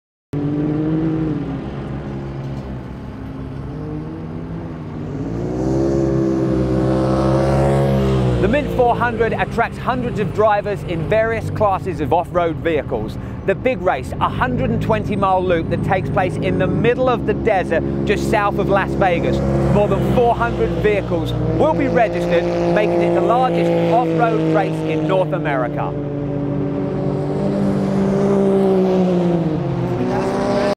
Paul Scally Hosts LIVE FROM THE 2018 MINT400 Las Vegas
LIVE SPORTS PRESENTER Paul Scally Presents LIVE From Las Vegas Desert MINT 400 THE MOST EXTREME DESERT RACE IN NORTH AMERICA